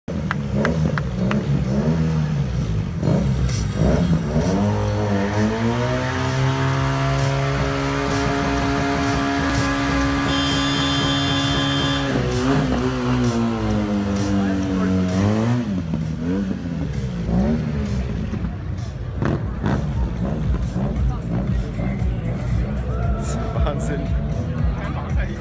Karl Maier bei BMW Days 2016
Karl Maier mit der Scrambler bei den BMW Days 2016 in Garmisch